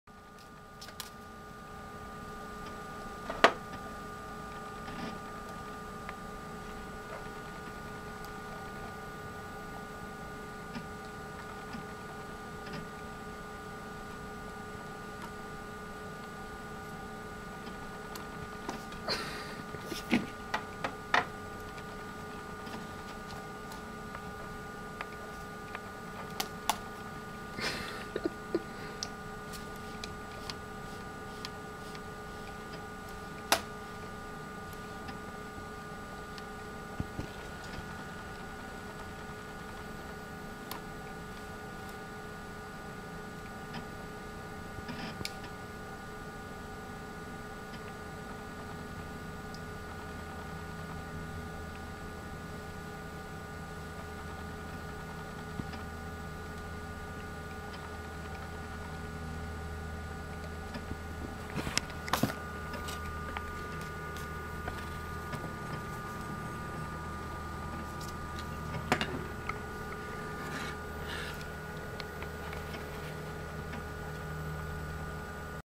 Cute Kitty chasing "mouse"
Adorable kitten I was baby sitting was sitting on my lap when she started to chase after the computer mouse!